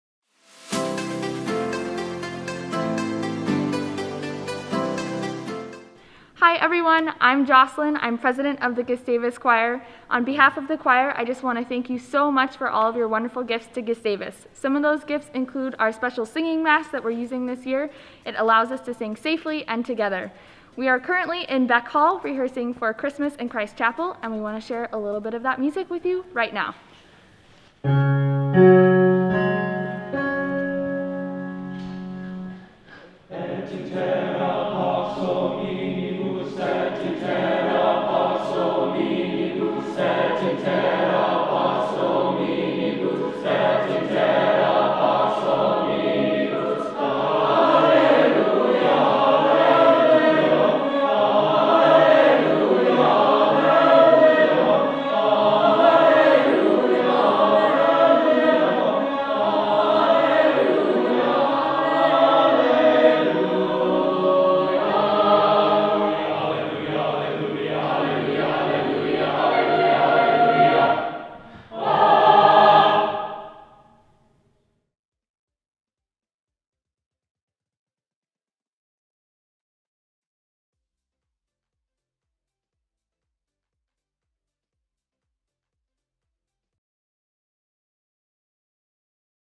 Give to Gustavus Day: Gustavus Choir Rehearsal
Thanks to generous donors, choir students at Gustavus have continued to make music together despite the pandemic.